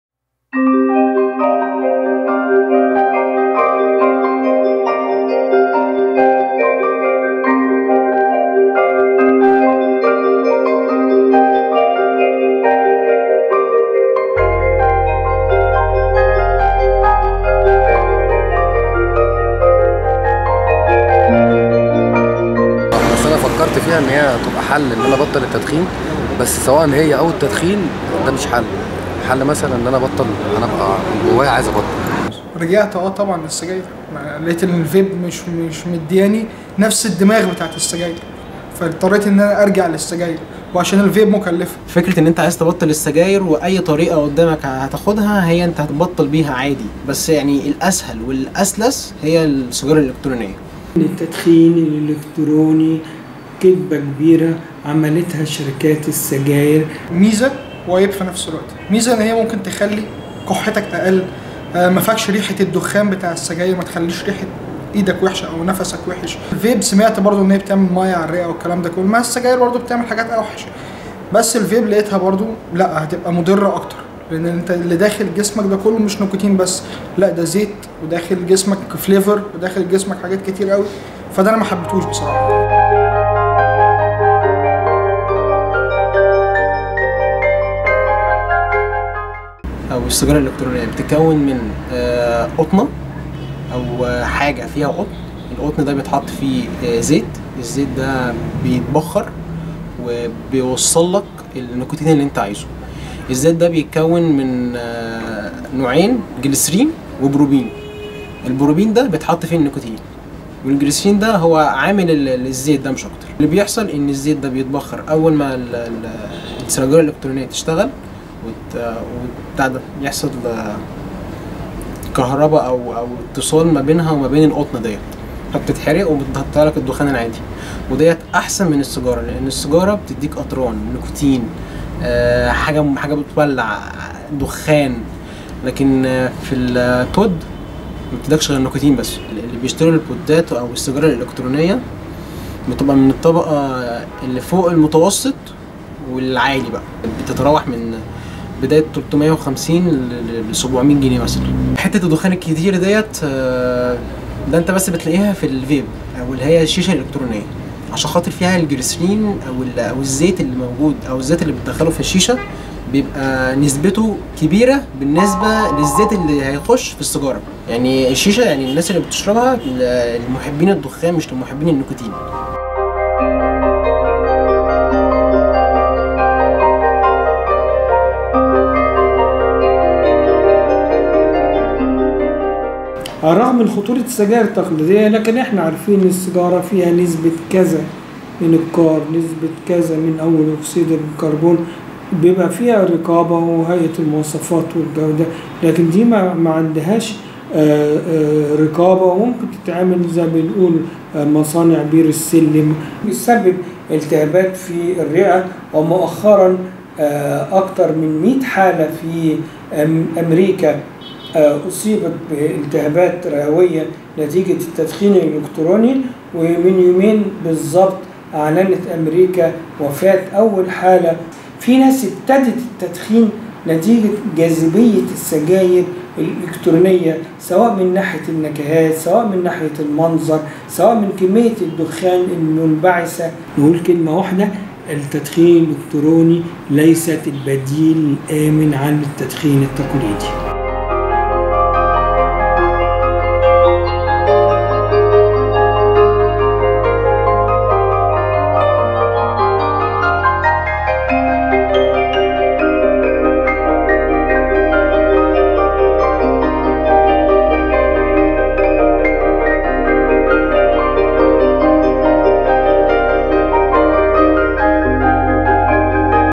مواطنون بعد أول حالة وفاة بسبب السجائر الإلكترونية: جربناها ورجعنا ندخن أكتر
جريدة الوطن
جريدة يومية مستقلة